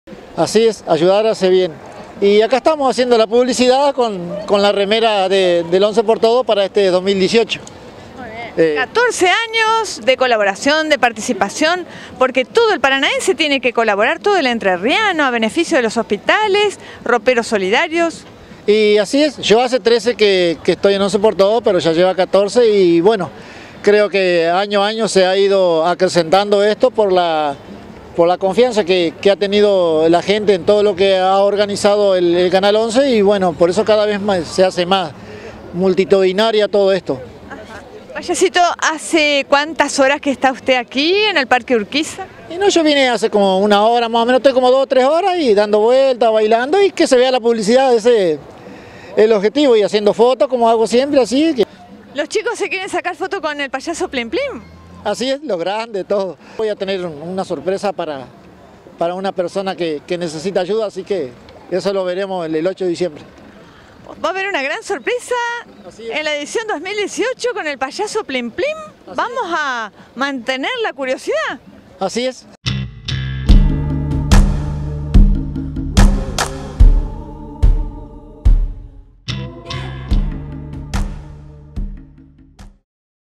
Plim Plim, el “Payaso Solidario” de Paraná y su promesa para Once por Todos
“Plim-plim” ya es un símbolo de la jornada solidaria Once por Todos. El “Payaso Solidario” de Paraná que cada año se suma a la jornada organizada por Elonce TV a beneficio de hospitales, ONGs, entidades, y escuelas.
Desde la segunda edición de Once por Todos, el payaso paranaense colabora con la jornada solidaria, y varios meses antes del 8 de diciembre, recorre los comercios de la Peatonal San Martín apelando a la solidaridad de los paranaenses.

La confianza de la gente
El año pasado logró reunir más de 23 mil pañales y en la 14º edición de Once por Todos, tiene nuevos desafíos. “Año a año, el Once por Todos, ha ido creciendo por la confianza de la gente en la organización de Canal Once y por eso, cada vez, se hace más multitudinario”, afirmó Plim Plim a Elonce.
Este domingo estuvo varias horas en la Costanera Baja de la capital entrerriana, bailando, sacándose fotos y saludando a los chicos. “El objetivo es que se vea la remera del Once por todos para hacer publicidad y que la gente sepa que ya se viene”, afirmó.

Una sorpresa
Para la 14º edición del Once por Todos, el “Payaso Solidario” de Paraná contó que el beneficio de su colecta solidaria, se destinará como todos los años a la jornada organizada por Elonce TV, pero también, prometió “una sorpresa para una personita muy especial que también necesita ayuda. Lo vamos a decir el 8 de diciembre”, anticipó Plim Plim.